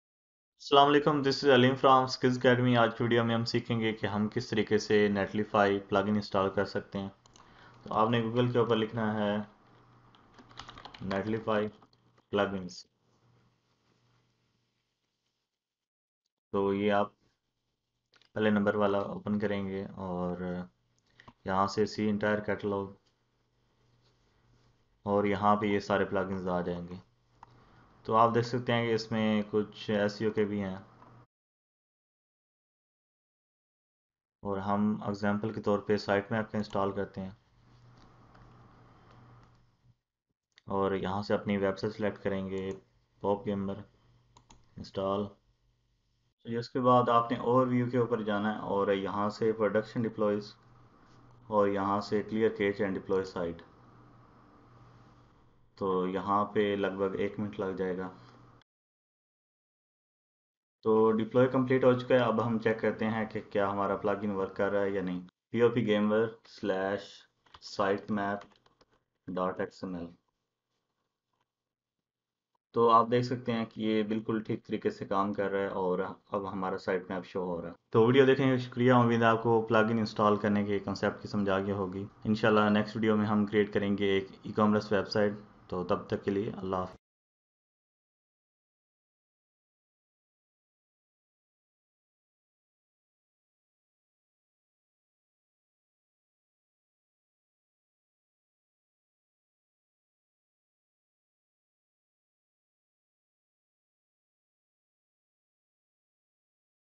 How to Install Plugins on Netlify Websites
To install a plugin on a Netlify website, you will need to follow these steps:

Open your Netlify project and go to the "Settings" tab.
In the "Build & Deploy" section, click on the "Edit settings" button next to the "Build plugin" option.
Click the "Install new plugin" button.
Search for the plugin you want to install and click the "Install" button next to it.
Configure any necessary options for the plugin and click "Save".

Please note that not all plugins are compatible with Netlify. Some plugins may require additional setup or configuration in order to work properly. If you encounter any issues while trying to install a plugin, it is recommended to consult the plugin's documentation or reach out to the plugin's support team for assistance.

💥💥Vieo Chapters💥💥
00:00 Universal SEO Tool
00:48 Building Dofollow Comment Backlink
03:50 Building Dofollow Forum Backlinks
14:32 Ending